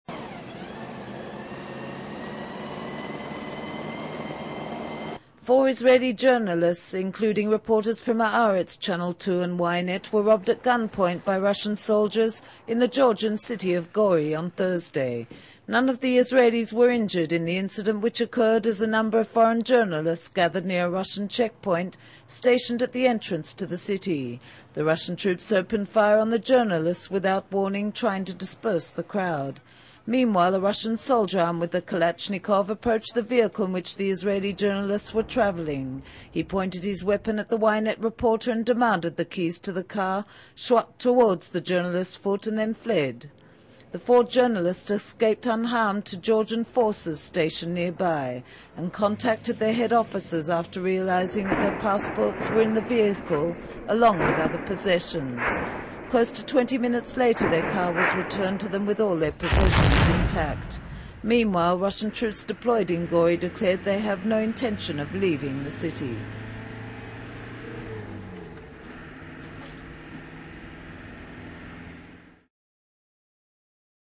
Gun Toting Russian Soldiers Rob Israeli Journalists In Gori
Four Israeli journalists, including reporters from Haaretz, Channel Two and Y-net,  were robbed at gunpoint by Russian soldiers in the Georgian city of Gori on Thursday. None of the Israelis was injured in the incident, which occurred as a number of foreign journalists gathered near a Russian checkpoint stationed at the entrance to the city. The Russian troops opened fire on the journalists without warning, trying to disperse the crowd. Meanwhile, a Russian soldier armed with a Kalashnikov rifle approached the vehicle in which the Israeli journalists were traveling. H epointed his weapon at the Ynet reporter and demanded the keys to the car, shot towards the journalis&#39;s foot and then fled in the car. T